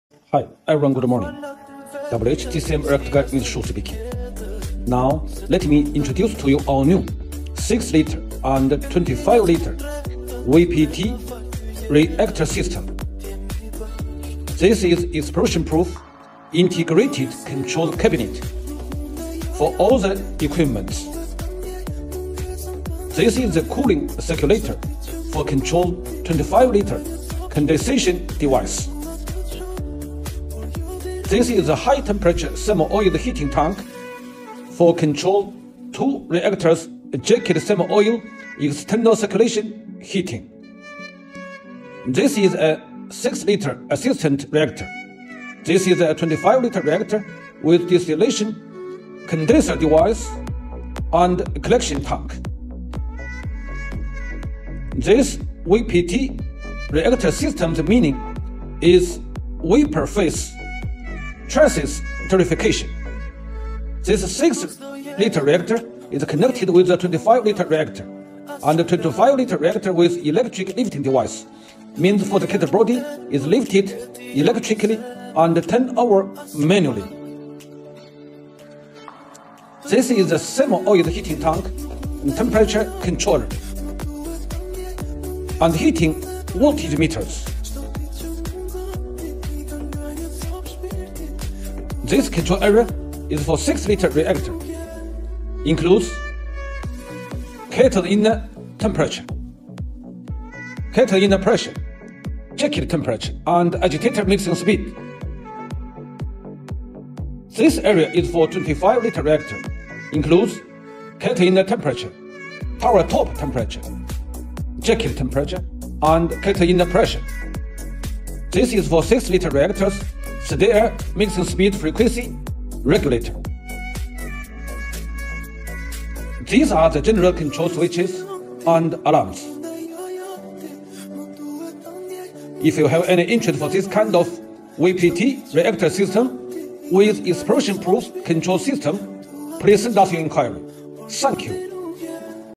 6L+25L VPT (Vapor Phase Transesterification) Reactor System made by pressure vessel supplier WHGCM
Product model:(6L+25L)VPT Reactor System
Note:all technical parameters and Nozzle ports can be customized

Anson Su (Reactor guy)-Sales manager of overseas center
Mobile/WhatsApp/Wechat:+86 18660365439